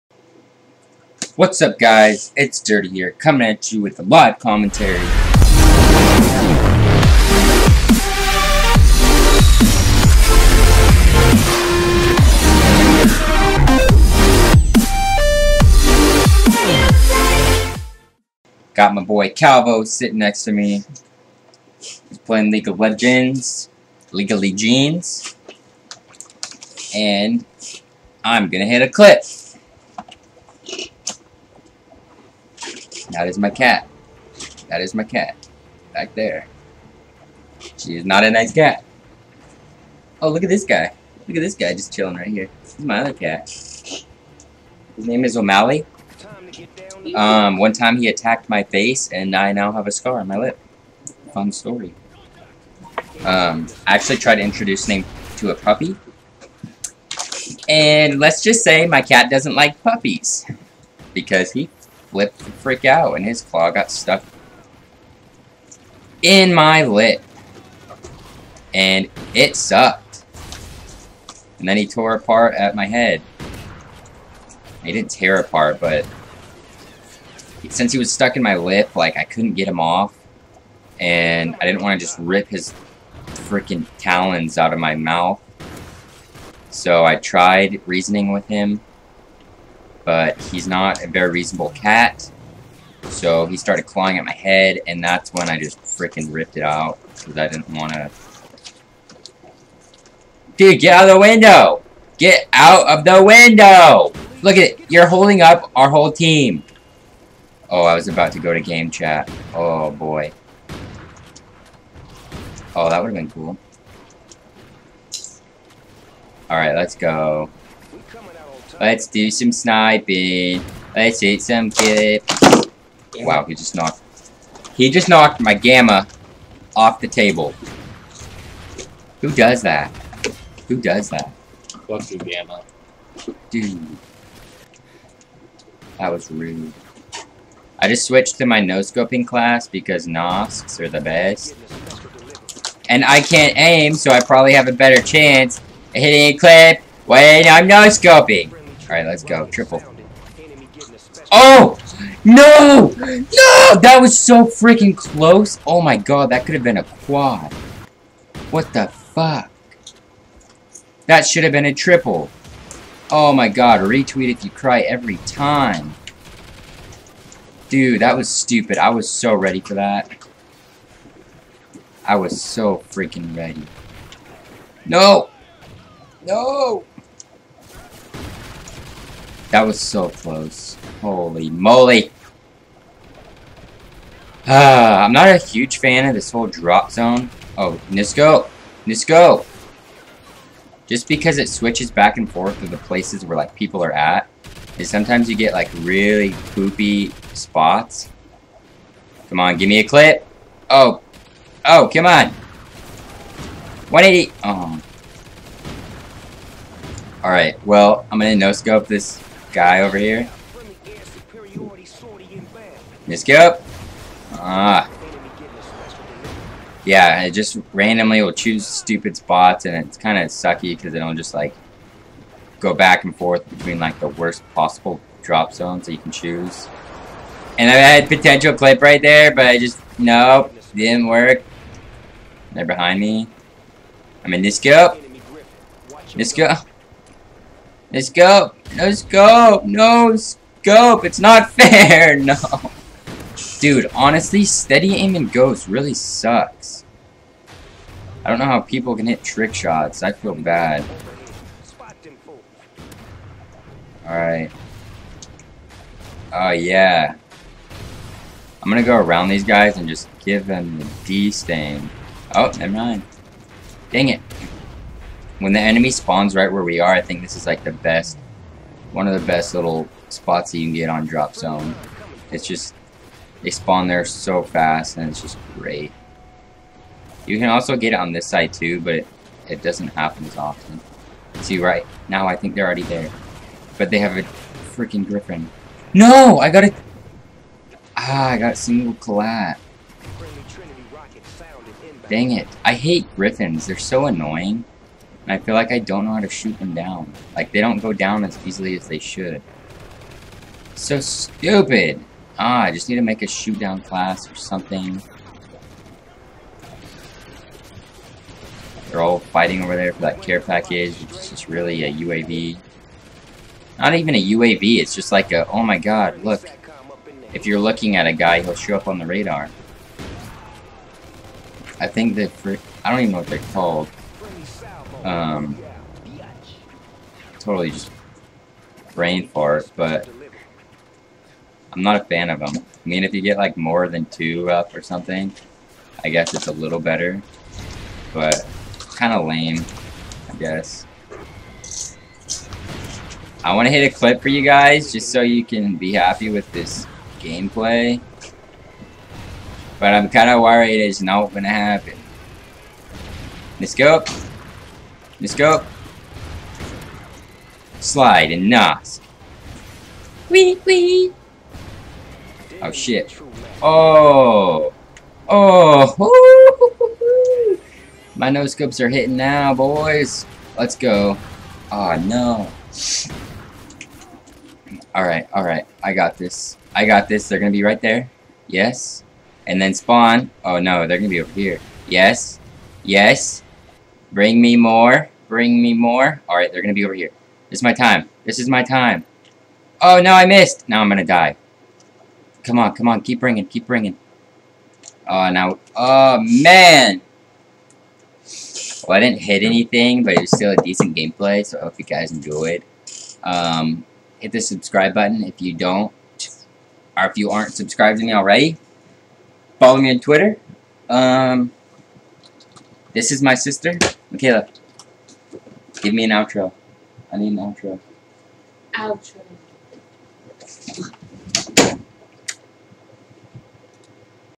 Just your Average Live Commentary - Dirty nV
I honestly have no idea why my audio messes up all the time /: I think it's my elgato.. The games skip and I dont know why.